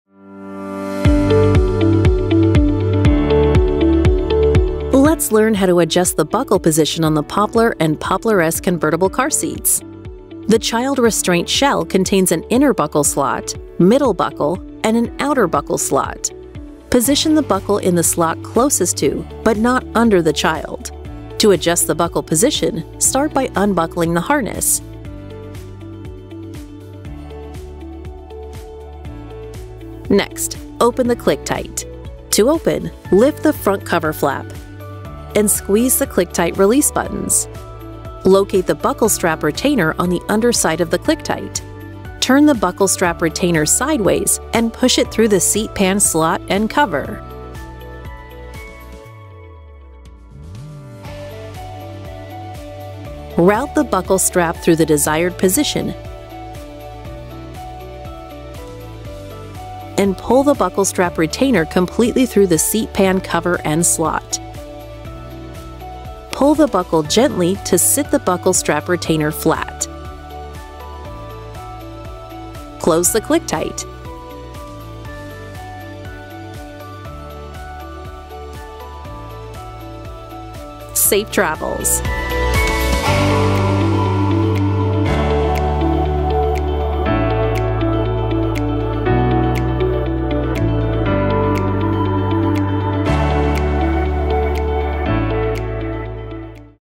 How to Adjust the Buckle: Poplar & Poplar S Convertible Car Seats
Ready to go with room to grow! Learn how to adjust the buckle position on the Britax Poplar & Poplar S convertible car seats. Safety starts with the right fit, so make sure you position the buckle in the slot that’s closest to but not under the child.

Always refer to your user manual for instructions and warnings. Questions? Call customer service at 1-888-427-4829.